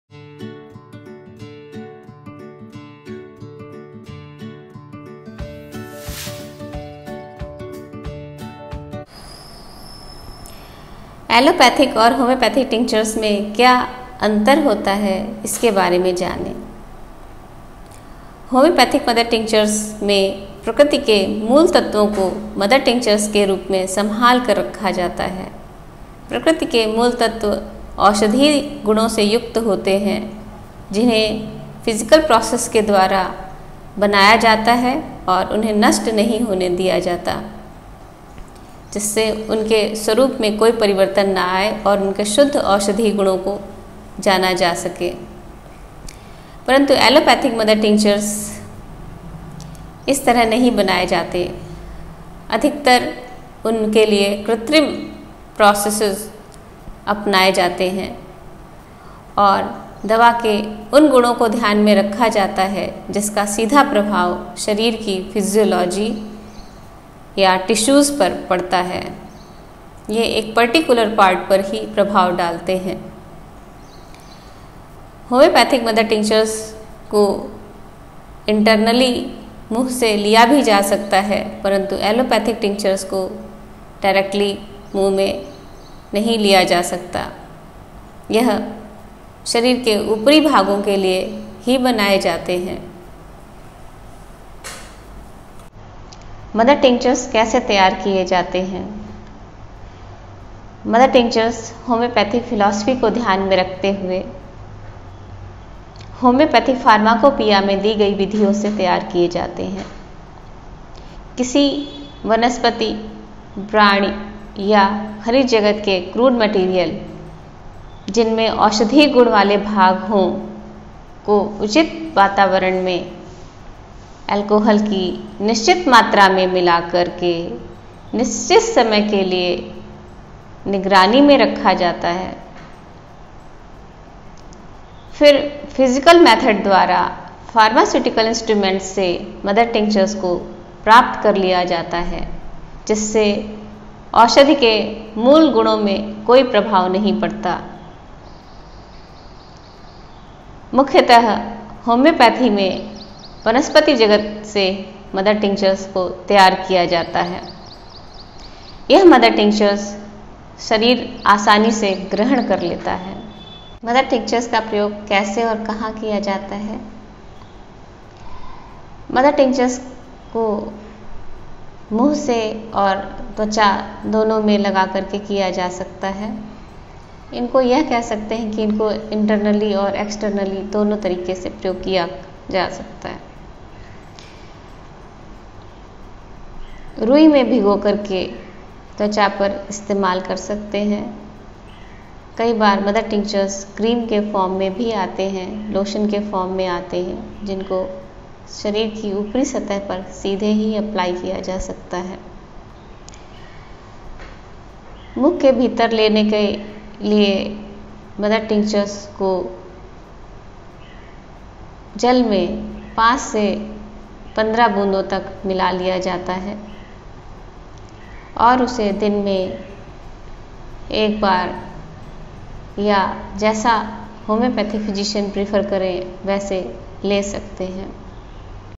HOMOEOPATHIC MOTHER TINCTURES : AN INTRODUCTION part-2/ होम्योपैथी में मदर टिंक्चस॔: एक परिचय(भाग-2)
Mother Tinctures in Homeopathy (part 2)
All about mother Tinctures
अब प्रश्न है की एलोपैथिक और होम्योपैथिक टिंक्चसॆ क्या आपस में कुछ समानता रखते हैं?
होम्योपैथिक मदर टिंक्चर -प्रकृति के मूल तत्वों को मदर टिंक्चर्स में संभाल कर रखा जाता है परंतु एलोपैथिक मदर टिंक्चर इस तरह से नहीं बनाए जाते उनमें अधिकांशत :ृत्रिम तरीके अपनाए जाते हैं और दवा के उन गुणों को ध्यान में रखा जाता है जिसका प्रभाव शरीर की फिजियोलॉजी या टिश्यूज पर पड़ता है।
प्रश्न मदर टिंक्चर्स कैसे तैयार किए जाते हैं ?
मदर टिंक्चर होम्योपैथिक फिलॉस्फी को ध्यान में रखते हुए होम्योपैथिक फार्मोपिया में दी गई विधियों से तैयार किए जाते हैं
किसी वनस्पति, प्राणी या खनिज जगत के क्रूड मैटेरियल जिनमें औषधीय गुण वाले भाग को उचित वातावरण में अल्कोहल की निश्चित मात्रा मिलाकर निश्चित समय तक निगरानी में रखा जाता है फिर फिजिकल मेथड द्वारा फार्मास्यूटिकल इंस्ट्रूमेंट्स से मदर टिंक्चर्स प्राप्त कर लेते हैं।
मुख्यतः वनस्पति जगत से मदर टिंक्चर्स तैयार होते हैं यह टिंक्चर्स को शरीर आसानी से ग्रहण कर लेता है।
प्रश्न मदर टिंक्चर्स का प्रयोग कैसे और कहां किया जाता है?
मदर टिंक्चर्स को ऊपरी त्वचा में रुई में भिगोकर
लगाने और भीतरी मुख से ग्रहण करके किया जाता है जिसमें ये अधिकतर 5 से 15 बूंदों तक जल में मिलाकर दिन में दो से तीन बार तक करते हैं।

आइए मदर टिंक्चर्स के इंटरनल और एक्सटर्नल यूजेज के बारे में जाने
फोड़े फुंसियों में जहां पर पूरे शरीर के रोम कूपों और तेल ग्रंथियों आदि में इंफेक्शन हुआ हो वहां ऊपर से लगाने पर मैग सल्फ Q का प्रयोग करते हैं।
मैगसल्फ छोटे-छोटे दानों में जिनमें बहुत अधिक जलन के साथ खुजली हो रही हो वहां पर लगाने को दिया जाता है।
पीने के लिए बेलिस पेरेन्नीस दी जाती है जहां पर रोम कूप और तेल ग्रंथियों में इन्फेक्शन पूरे शरीर में हो।
भारतीय औषधियों में ‐
अकाईरेन्थस एस्पेराQ (जहां साथ में पूरे शरीर में जलन हो) ,
अजाडायरेक्टर इंडिकाQ (अधिकतर पीठ पर महीन गर्मी के दाने अंधरी प्रिकली हीट),
सिफैलेंड्रा इंडिकाQ (डायबिटीज में कार्बंकल होने पर ),
जिमनेमा सेलवेस्ट्रे Q ( डायबिटीज में फोड़े ार्बंकल ) का प्रयोग करते हैं।
इन्हें तीन से 5 ड्रप जल में तीन से चार बार पीने को दिया जा सकता है।